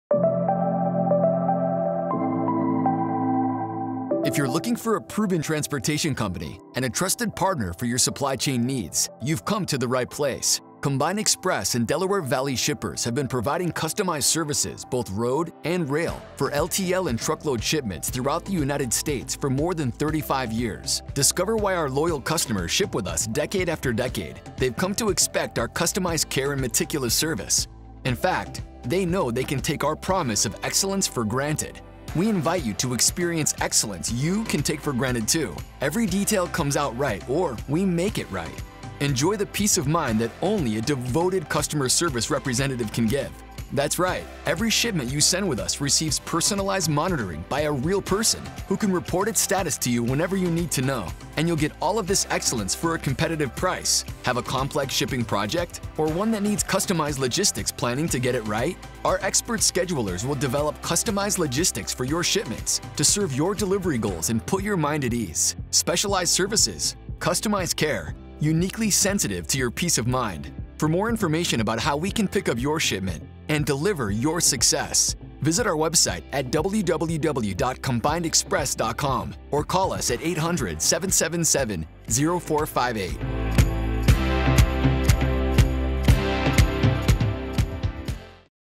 Freight Shipping Solutions by Combined Express
Combined Express & Delaware Valley Shippers
If you’re looking for a solid shipping company, a trusted partner for your freight transport needs, you’ve come to the right place!

Combined Express and Delaware Valley Shippers offer trusted, customized over-the-road and intermodal LTL and Truckload services, shipping throughout the United States … serving new and long-time customers for more than 35 years.

For more information about how we can pick up your shipment – and deliver your success – visit our

Corporate Office:

Combined Express, Inc.
3685 Marshall Lane
Bensalem, PA 19020